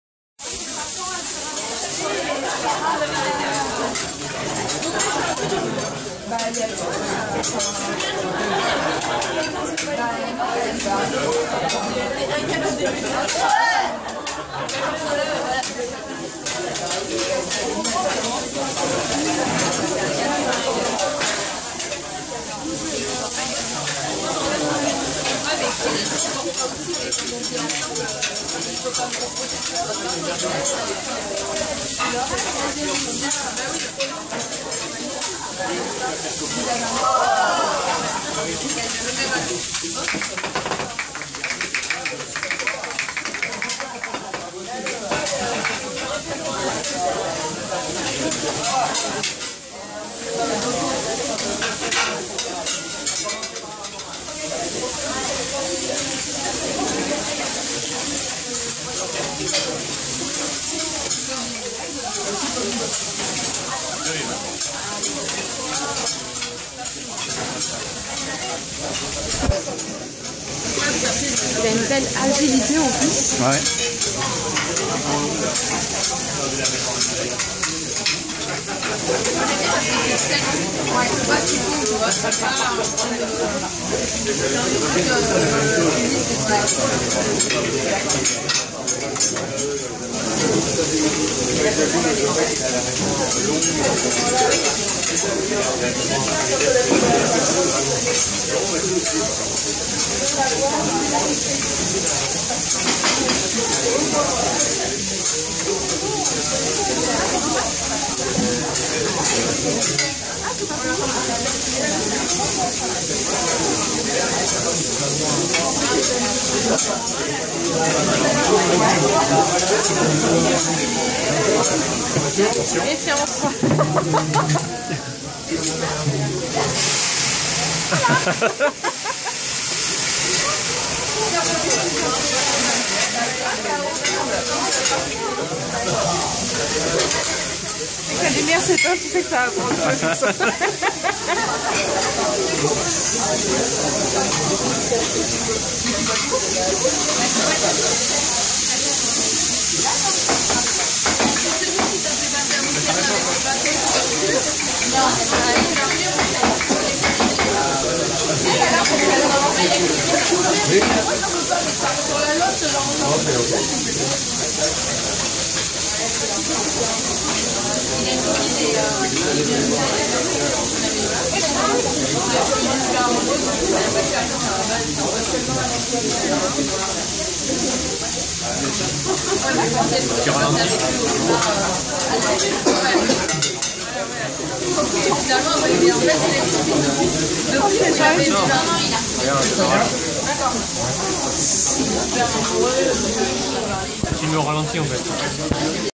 Teppanyaki chez Inagiku
Teppanyaki chez Inagiku, paris 5eme

Préparation Gambas et st Jacques